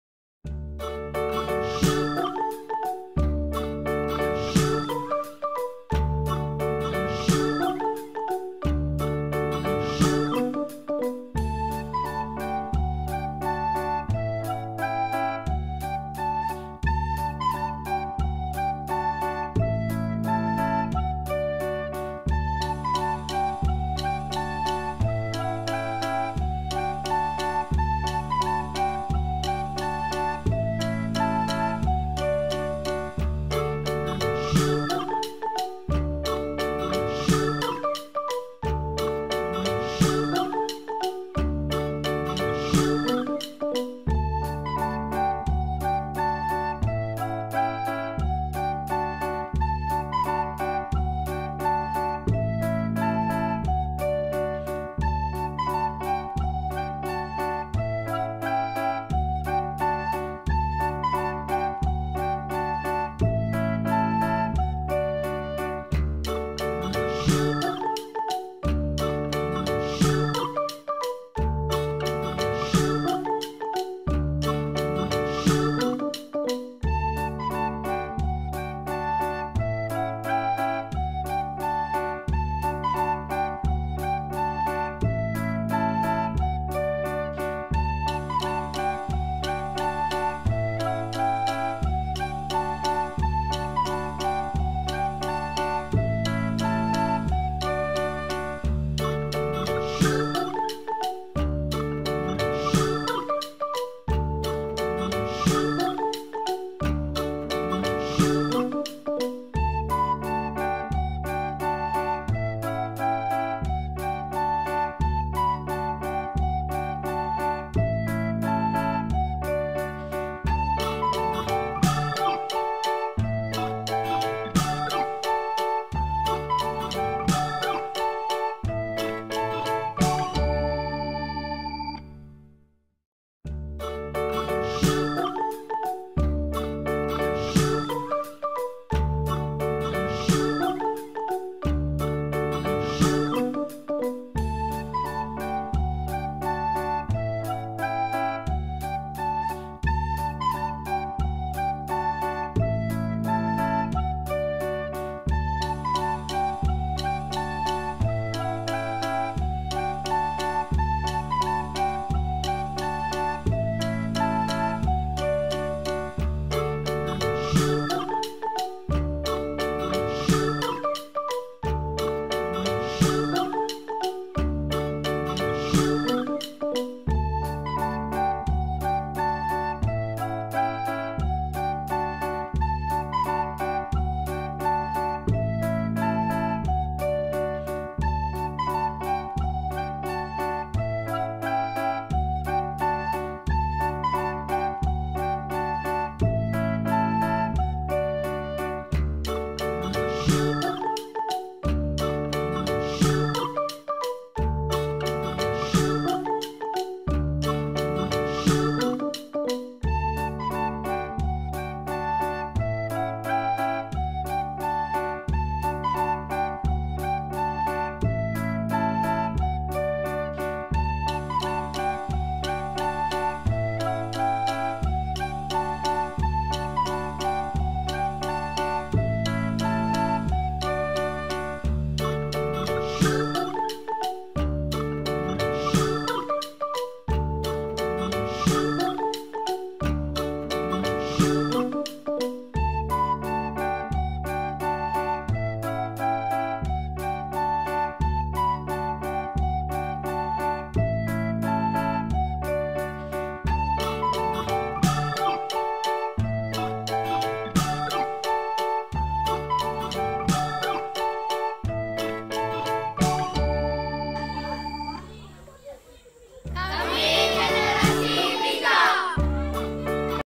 KemGenerasiBijak HDRF
Anjuran HDRF Malaysia
Kem bagi anak2 Emas tajaan HDRF. Berlansung pada 18-20 Mac/2018 di Hulu Langat.